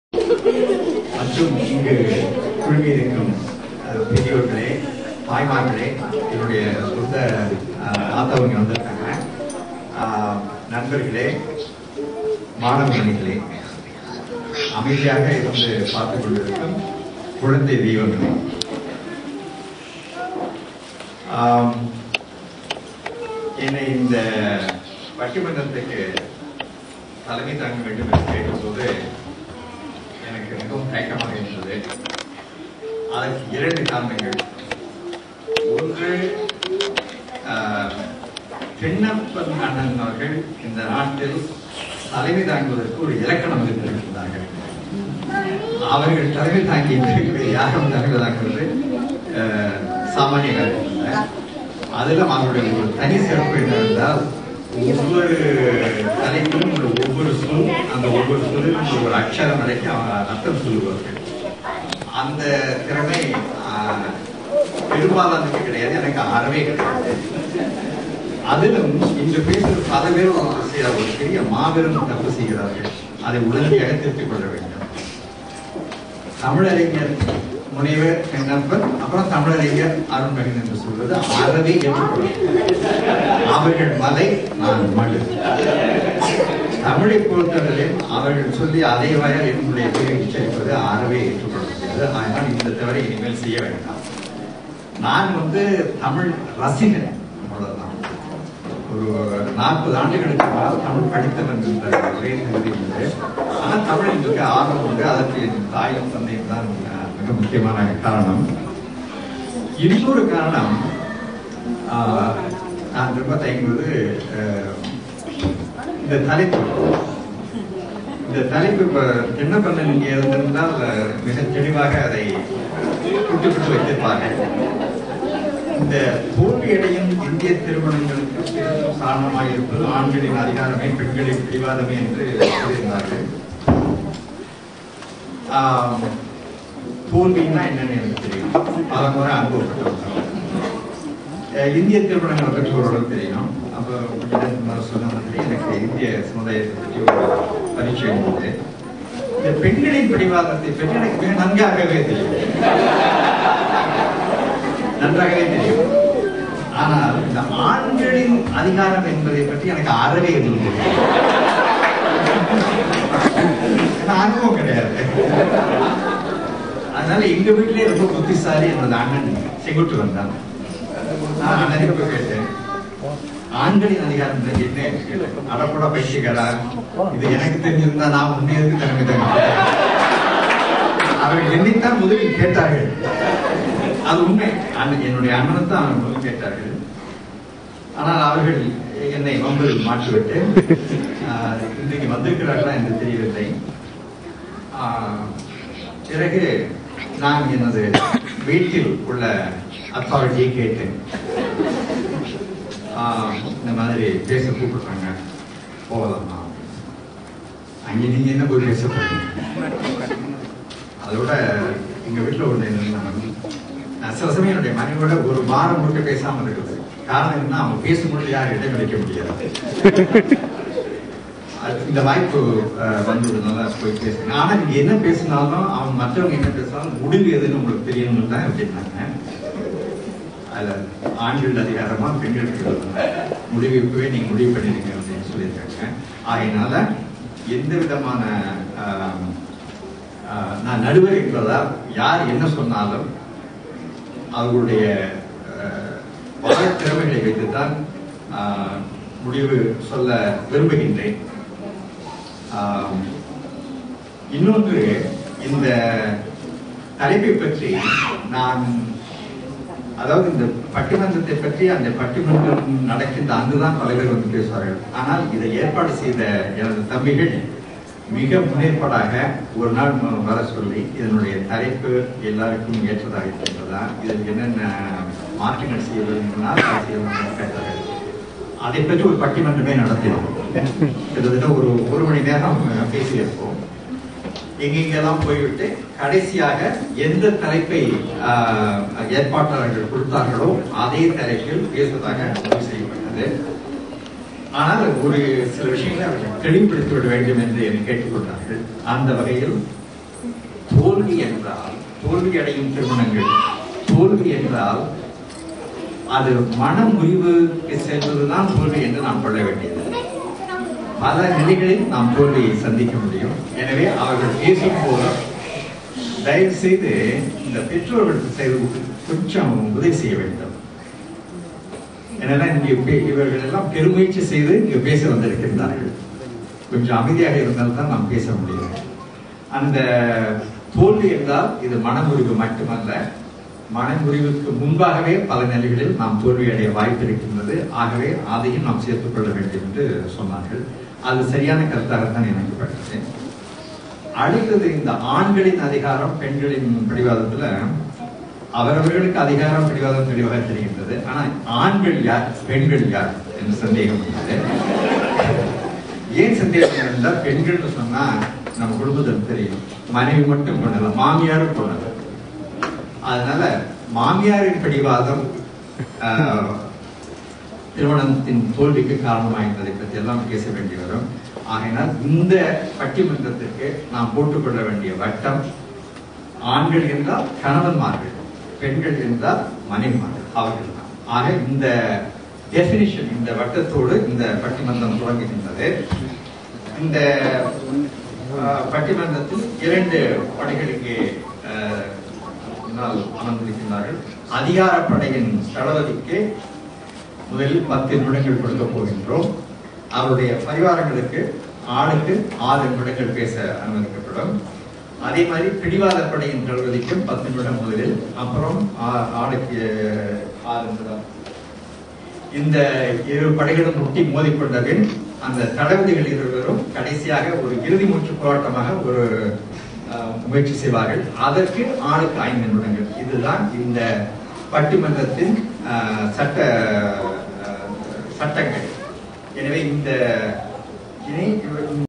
Arun Mazhinan (நடுவர்)
சிங்கப்புர் நகரத்தார் சங்கம் நடத்திய இலக்கிய விழாவும் போது நடந்த பட்டிமன்றம் நிகழ்ச்சியில் இருந்து சில காட்சிகள்.